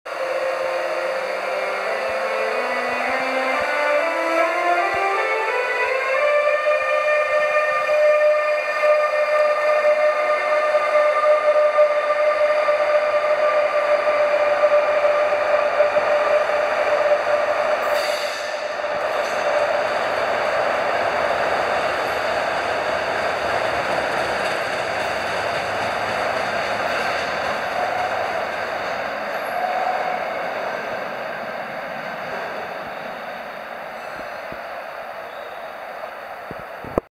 【オーストリア】駅構内に響き渡るドレミファ・インバーター
ウィーン空港駅を発車するシティエアポートトレイン。
ウィーン空港駅構内にドレミファ・インバーターが響き渡ります。